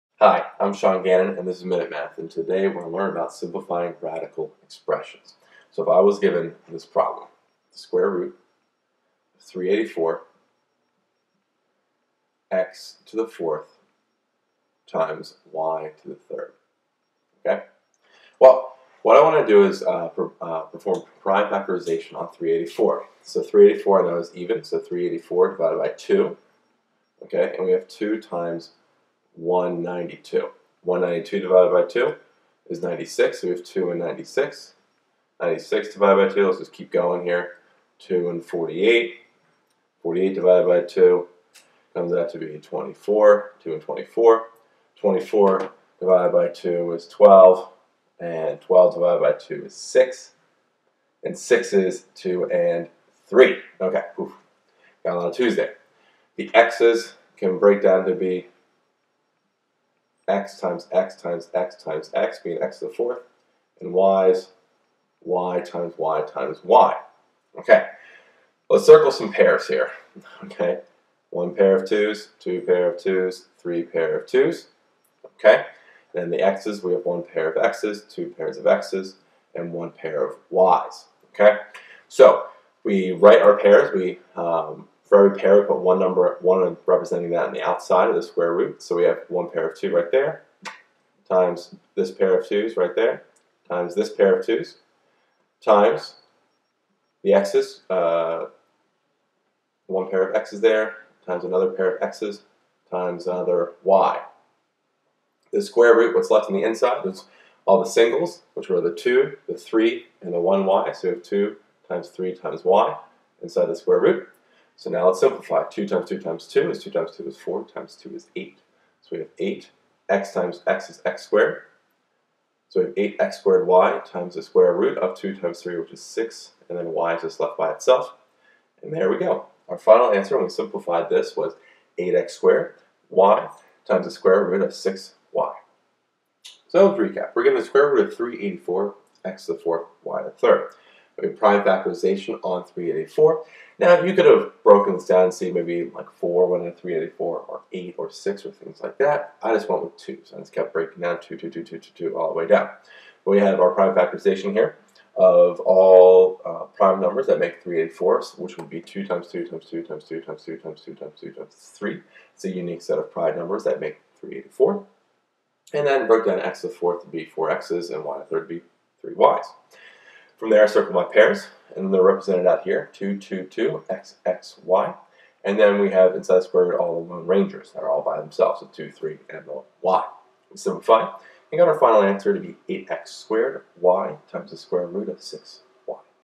Simplify √(384x^4y^3)
In this math video lesson on Simplifying Radical Expressions, I simplify the square root of 384x^4y^3.